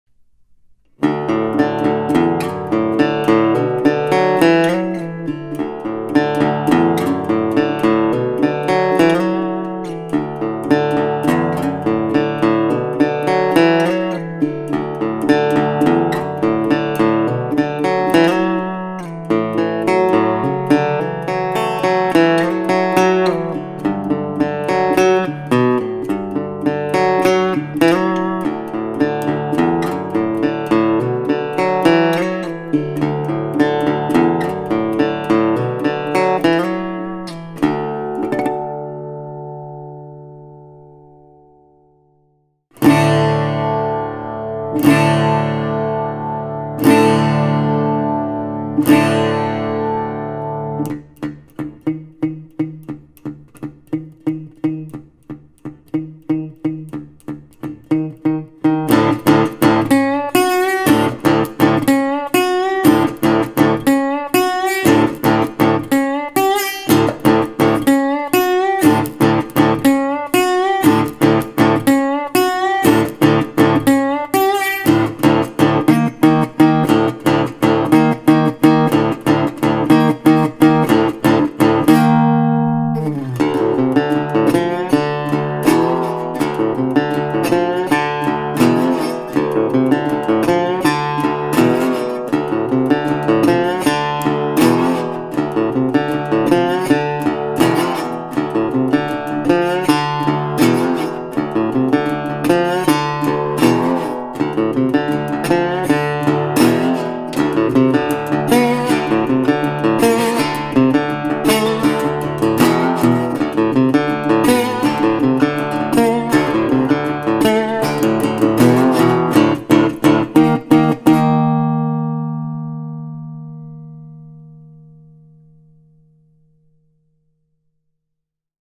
The Parlor Metal Body Resonator
The Recording King Parlor Metal Body Resonator resonator puts traditional reso style into a small body design that packs a punch.

True resonator tone begins with a classic nickel-plated bell brass body and RK’s own full-size hand-spun 9.5" cone, the same one used in RK’s best-selling Swamp Dog resonators.

Thanks to the smaller body size and 24-¼” scale, the Parlor Resonator has a different voice from other metal body instruments. It has all the swampiness and midrange of classic biscuit-style metal bodies, but the unique design adds more treble shimmer, giving it extra top-end clarity.

Recording King guitars never cut corners on tone, and the Parlor Resonator delivers classic sound, tons of vibe and roadworthy reliability, whether you’re just discovering resonator, or you’re a professional player.

• Nickel-Plated Bell Brass Body
• Hand-Spun 9.5” Cone
• Mahogany Neck, 24-¼” Scale, 1-¾” Nut Width
• Maple/Ebony Biscuit Bridge